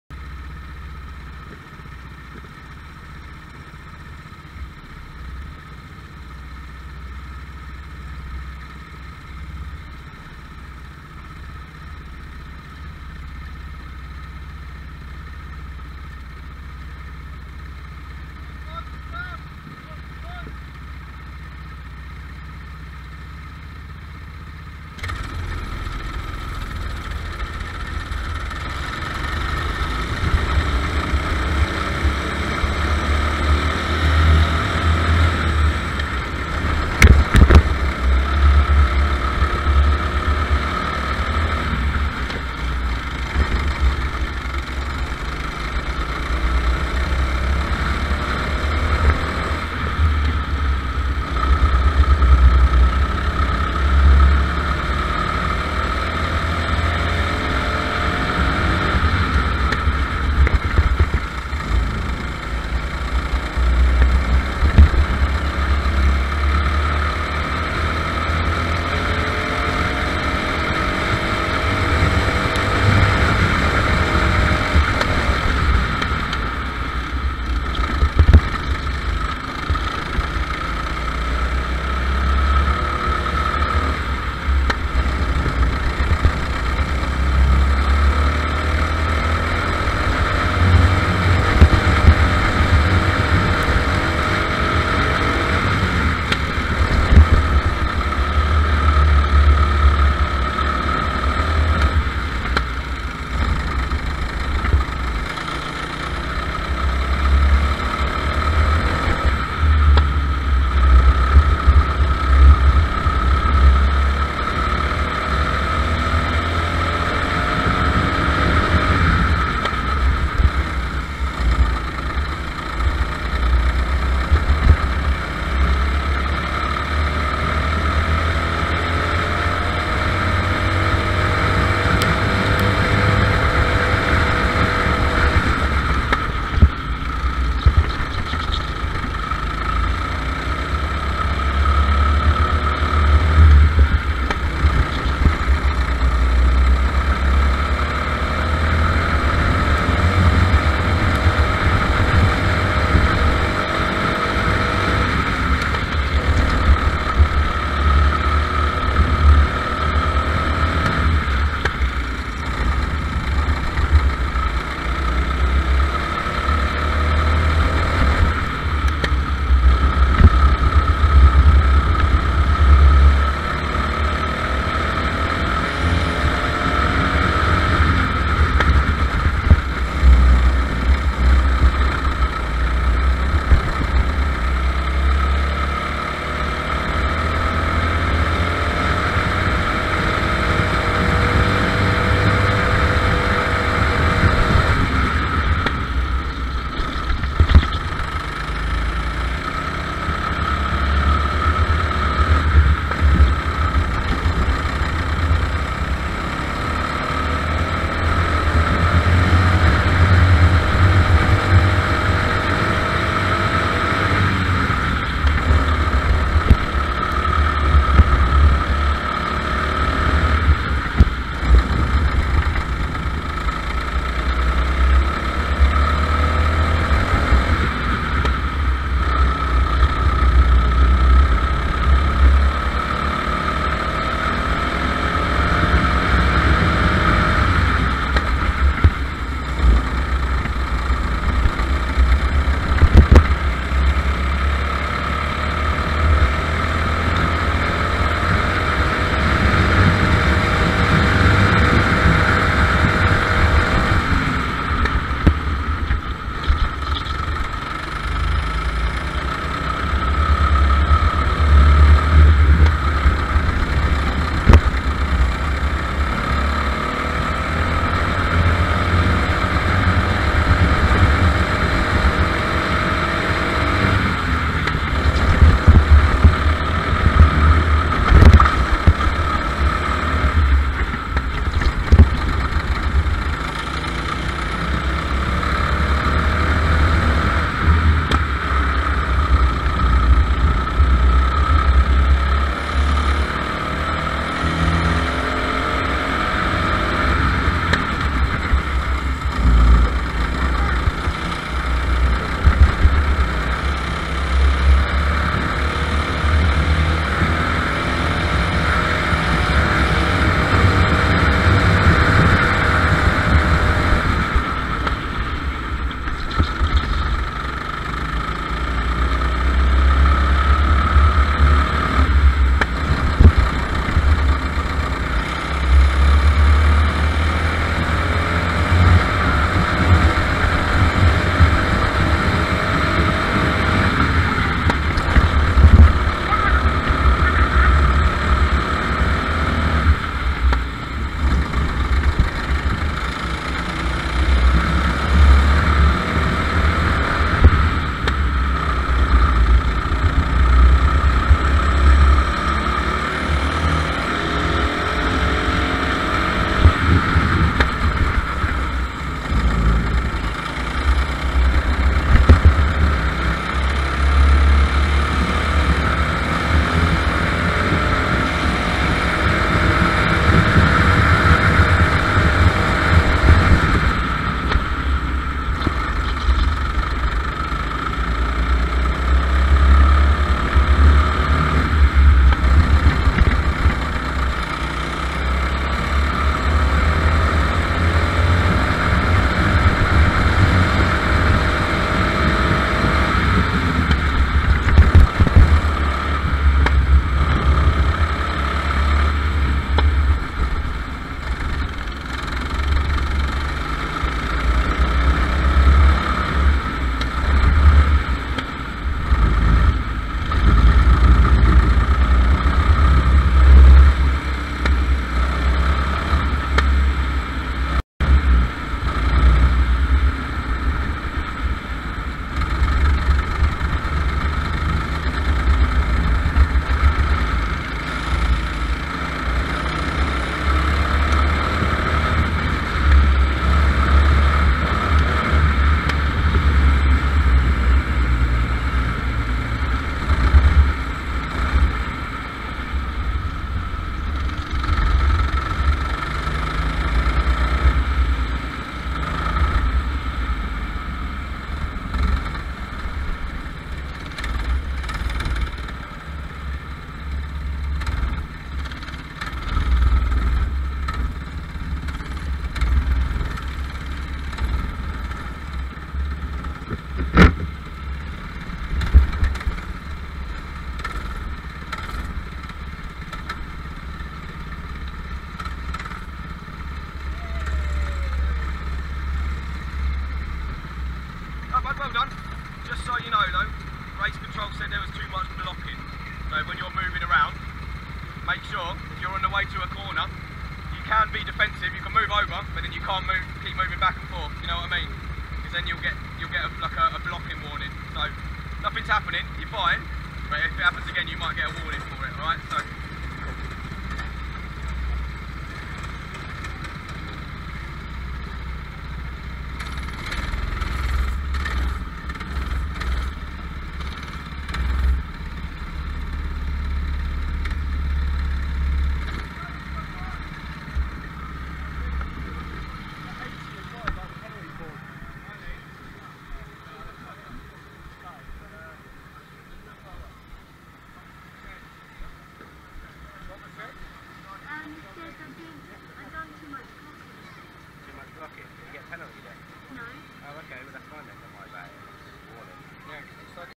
Round 8 Heat 1
4th place grid start with a 2nd place finish 59.088s best lap